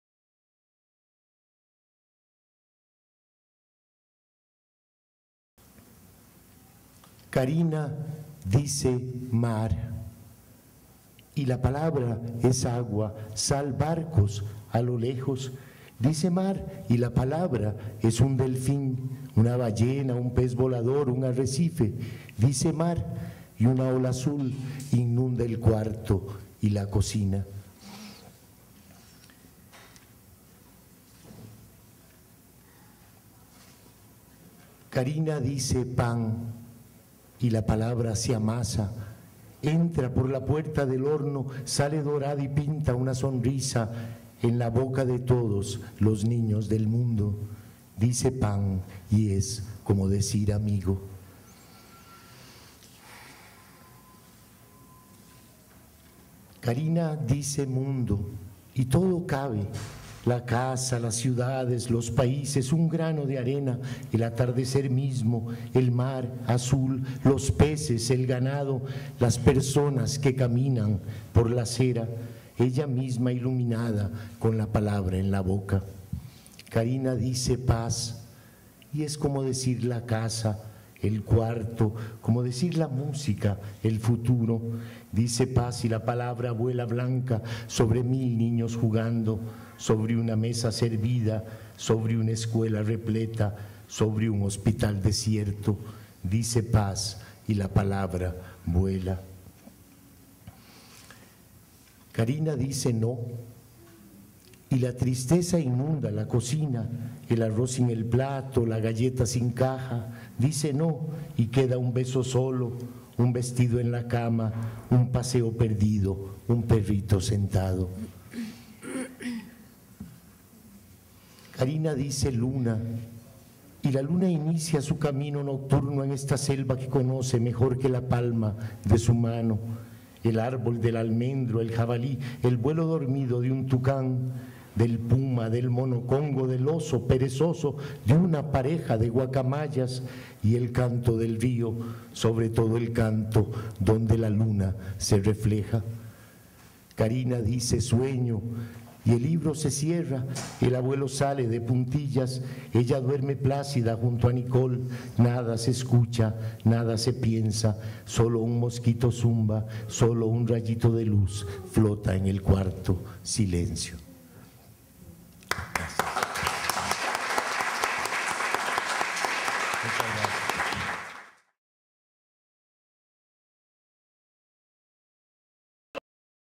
Karina dice sus primeras palabras (Rodolfo Dada, Costa Rica)
Memoria Audiovisual Festival Internacional de Poesía de Medellín. Rodolfo Dada nació en San José de costa Rica en 1952. Poeta y escritor. Integrante del grupo coordinador del Festival Internacional de Poesía de Costa Rica. Escritor de literatura infantil. A nivel de la poesía en el 2004 publicó el libro Cardumen; en esta publicación , en la primera parte, libros inéditos escritos desde la década de los setenta hasta el año dos mil tres, los cuales son: Cardumen (2003); Fotografía en Sepia (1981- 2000), Sobremesa (1984), Pequeño poema Colorado (1977) y Cuajiniquil (1975). En ese conjunto de obras se deriva de la temática del paisaje marino, selva y arena a una nostalgia profunda de los poblados, la infancia y los instantes detenidos en las fotos; luego, sin perder el hilo conductor de la naturaleza y el mar, hace transición  a  un mundo infantil mucho más marcado, en el que el poeta escribe en dirección a los niños, con historias y episodios coloridos que muestran los poemas como un dibujo coloreado con crayolas, historias en la que la naturaleza devuelve personajes casi mágicos llevados todos por un ritmo sólo comparable con los sonidos de las noches en el trópico.

Lectura de poemas en el 21° Festival Internacional de Poesía de Medellín, en la Casa de la Lectura Infantil de Comfenalco, el domingo 3 de julio de 2011.